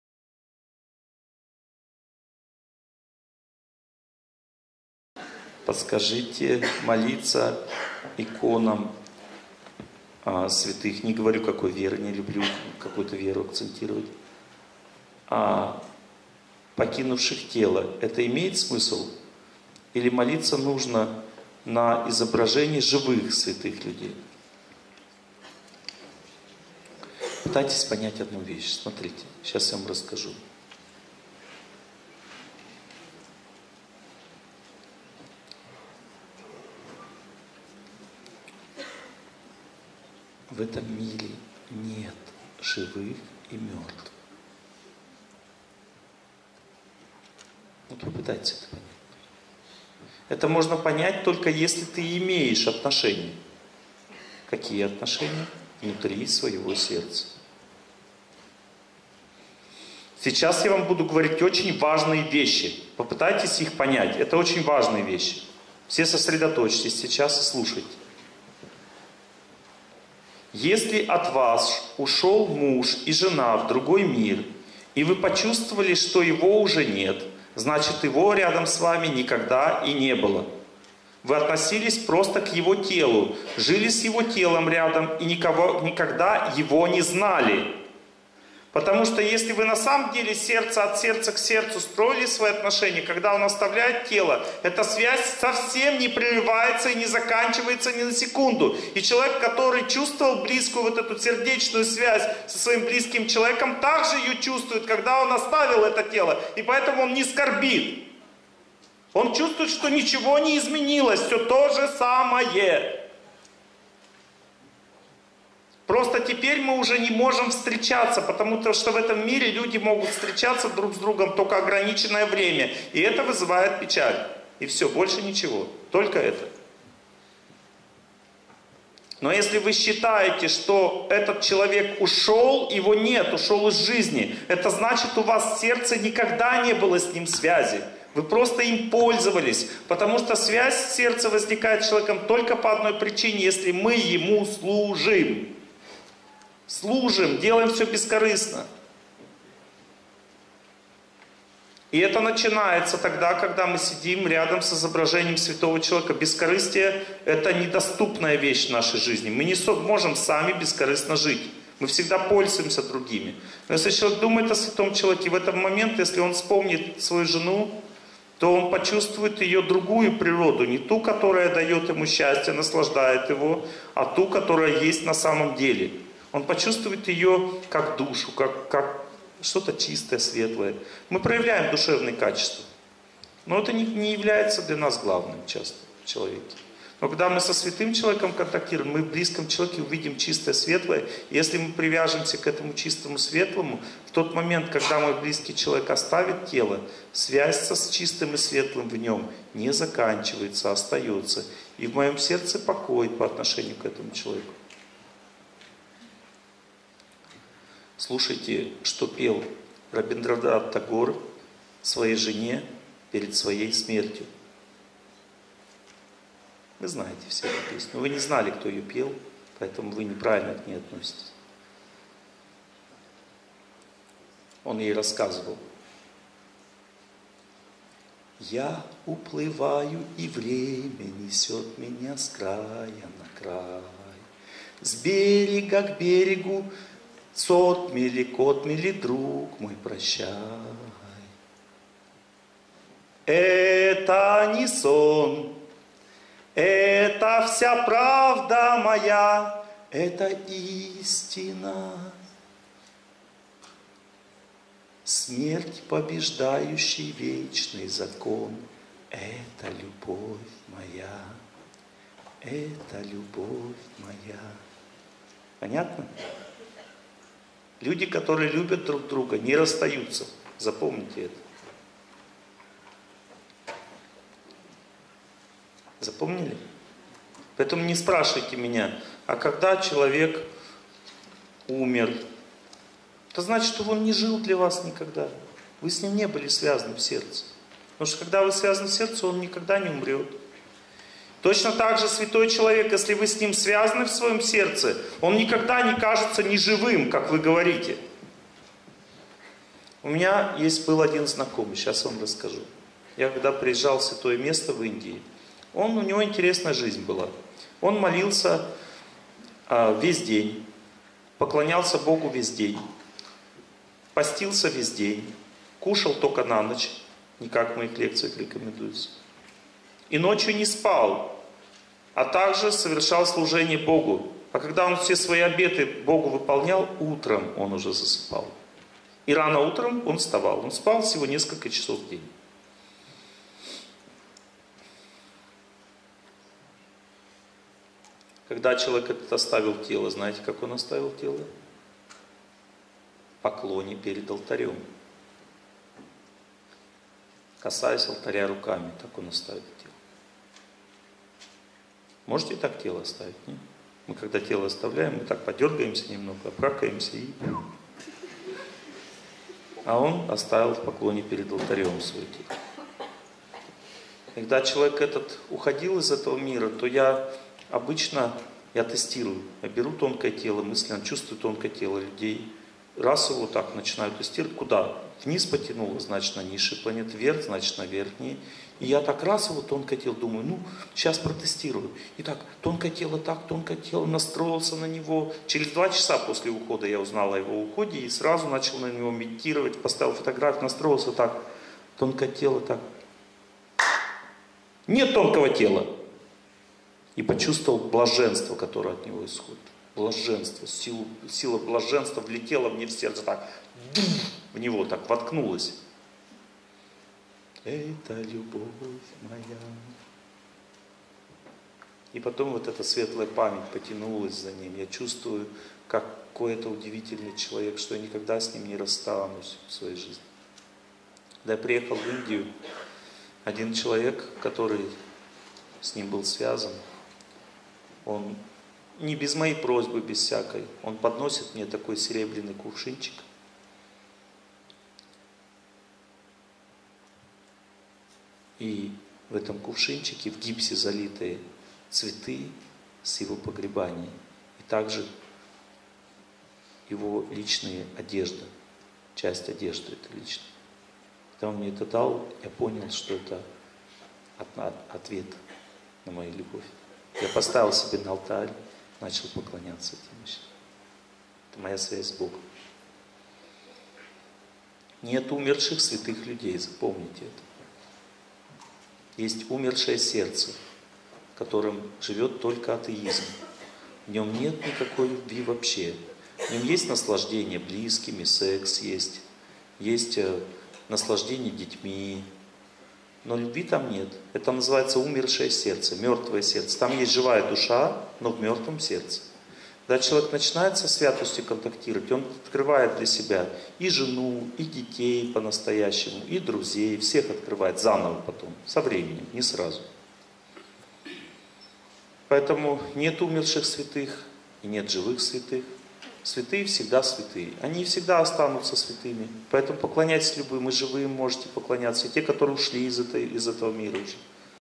Нет умерших святых людей
В этом видео Олег Геннадьевич Торсунов рассказывает о святых людях.